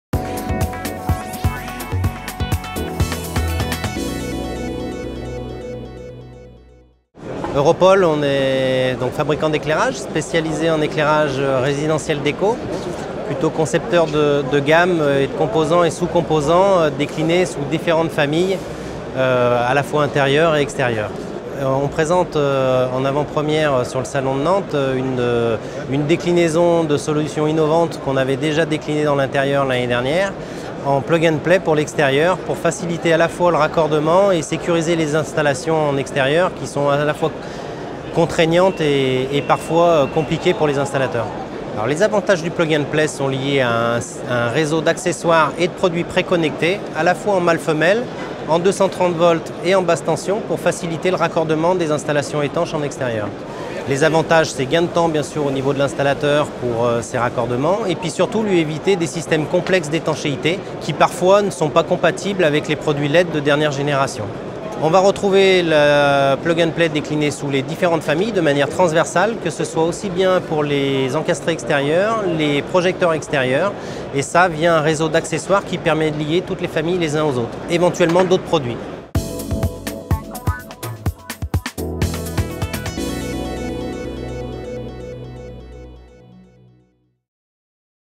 Focus EuroPole - Une solution “plug and play” pour l’éclairage extérieur
Ce Focus s’intéresse à la solution "plug & play" chez Europole pour l’éclairage extérieur. Cette solution facilite notamment le raccordement des équipements étanches et permet donc de gagner du temps au moment de l’installation.